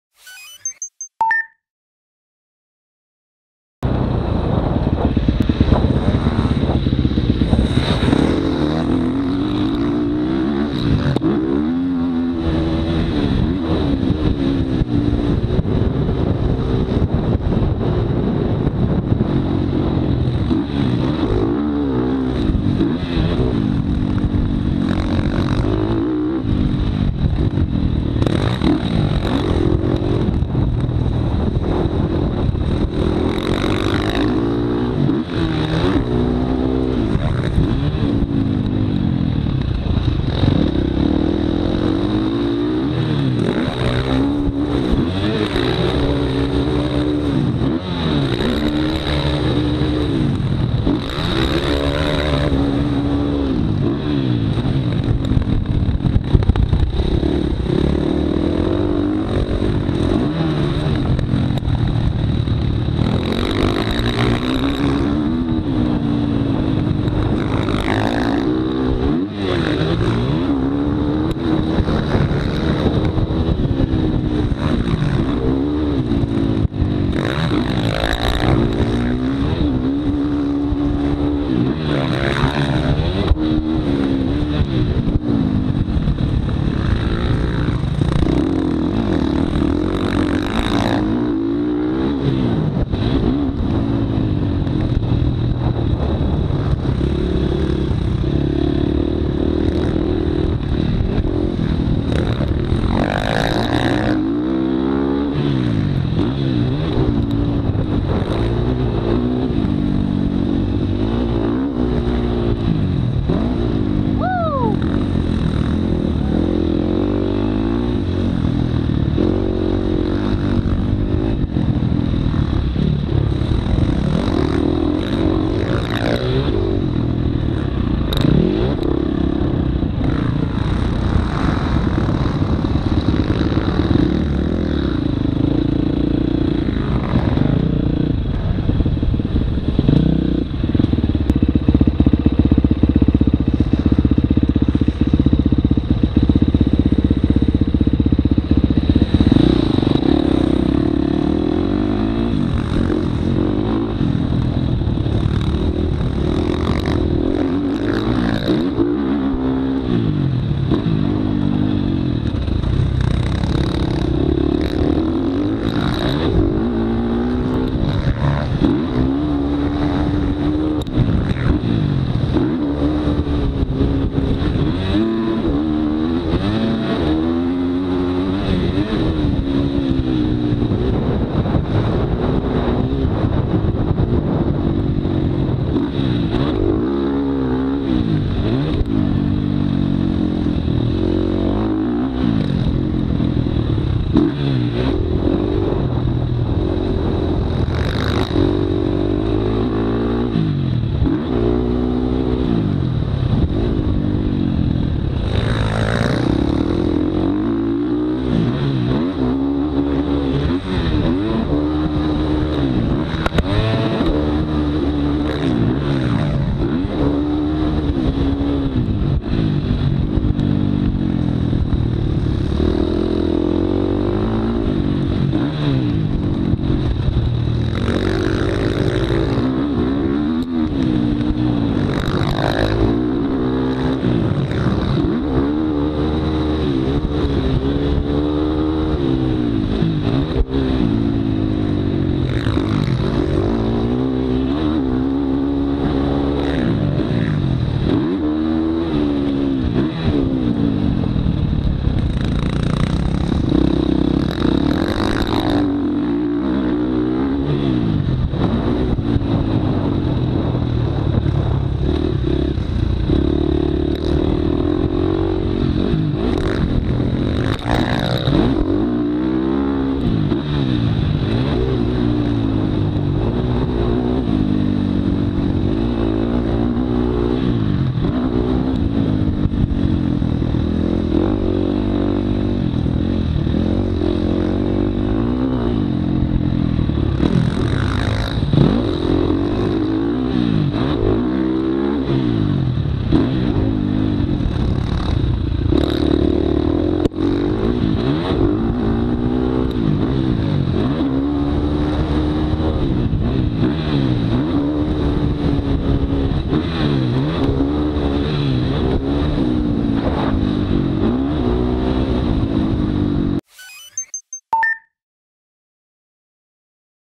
10-17-2020 Dtown riding with Joey
Good 2nd day back on the bike and was pushing it pretty good trying to catch Joey.